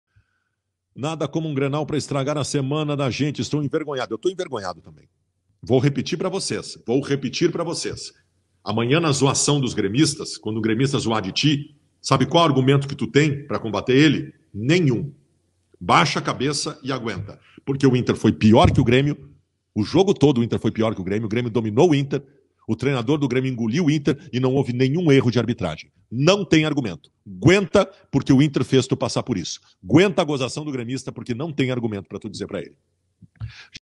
Baldasso: nada como um grenal pra começar mal a semana
Neste video veremos um desabafo do jornalista colorado fabiano Baldasso: nada como um grenal pra começar mal a semana

Para mais entretanimento
Inscreva se no canal do preto